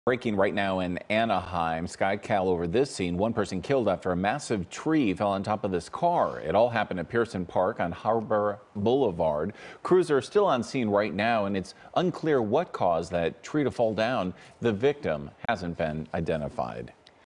One dead after massive tree falls onto car in Anaheim
The massive tree fell just before 2 p.m., killing one person inside of a vehicle near Anaheim Park on Sunday.